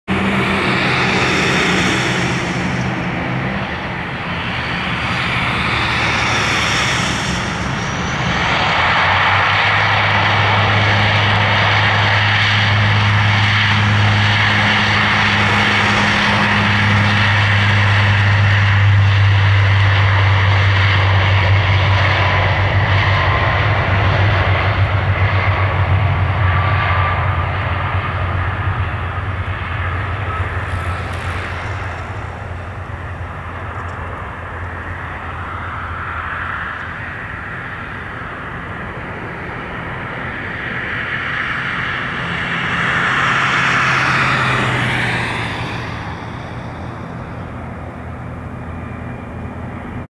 Boise State Broncos Alaska Airlines Q400 [N437QX] Takeoff Portland Airport (PDX)
Flight: QXE2458 PDX-BZN